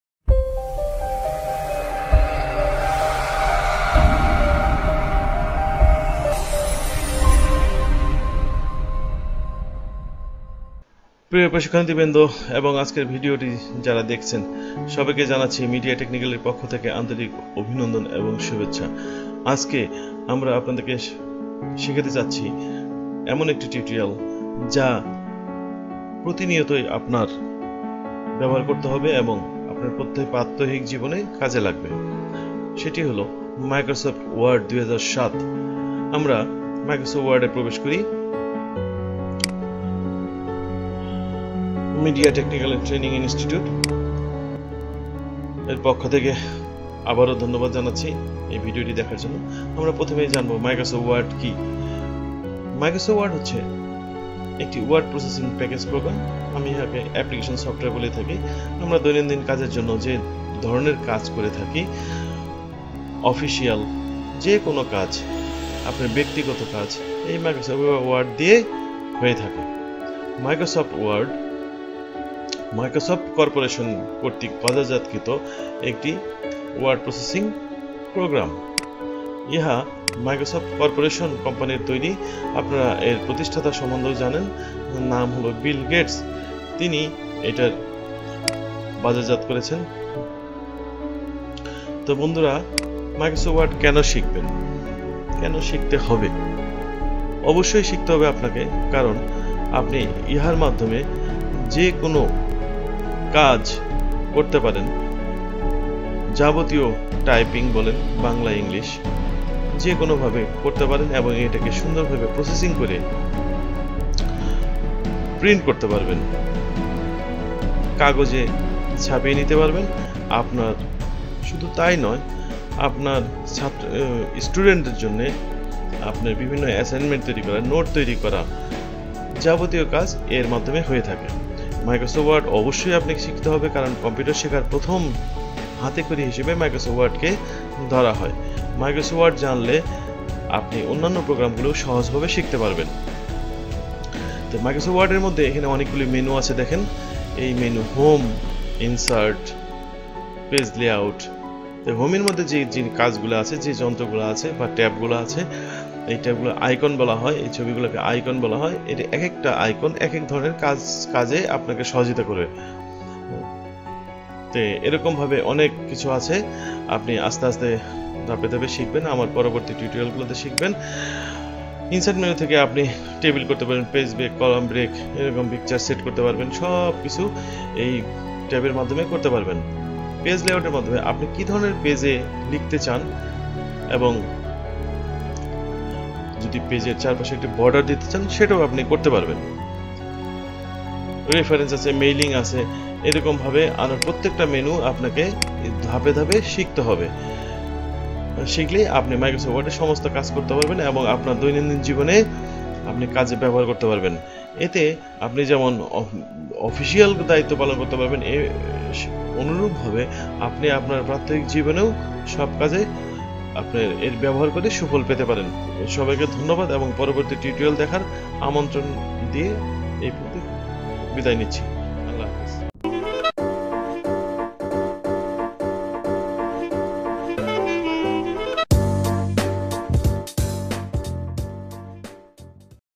Beginners Guide to Microsoft Word 2007 & 2010 in Bangla. Lesson-1.
Microsoft word is very much essential for every kind of people. Those who are mostly educated and work in office then they have to more important in this software.

How to#Beginners Guide to Microsoft Word#Mediattc